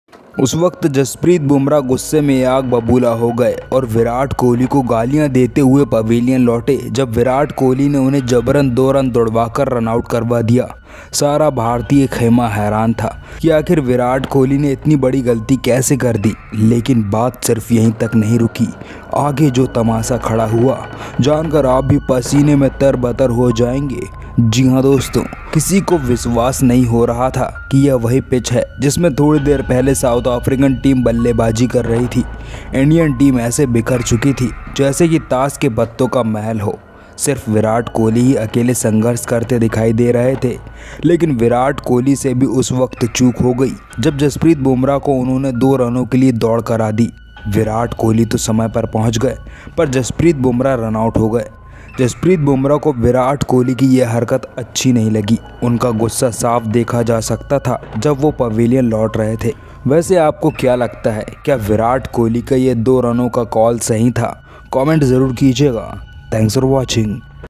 Huge fight between Jasprit Bumrah and Virat Kohli after this shambolic run out in Ind vs SA test
Huge eruption between Jasprit Bumrah and Virat Kohli after this shambolic run out in India vs South Africa 1st Test at Centurion cricket stadium.

Jasprit Bumrah was playing well but suddenly Virat Kohli made him run out against South Africa 1st test. Jasprit Bumrah couldn't control his emotions and exploded on Virat Kohli after run out.

jasprit bumrah,
virat kohli,
run out,
ind vs sa test,
virat kohli jasprit bumrah,